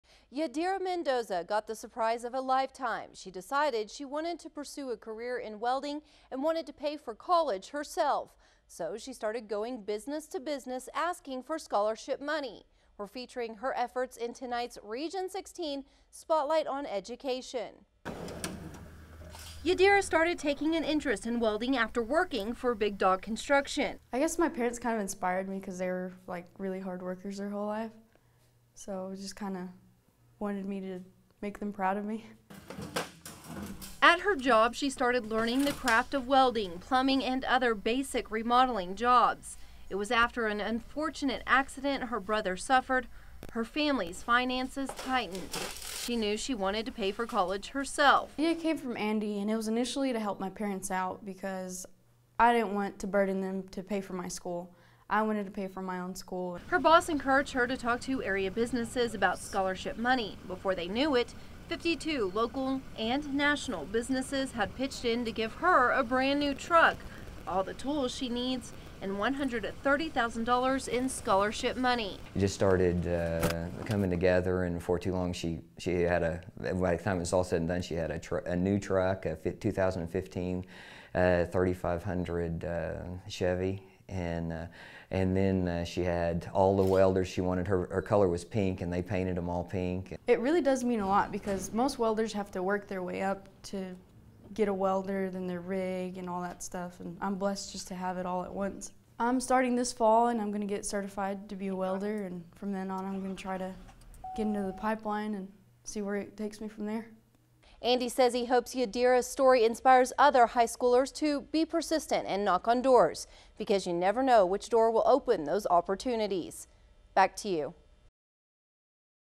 Gruver HS graduate proves hard work pays off
KVII-TV Pronews 7 Amarillo report by Lindsey Stiner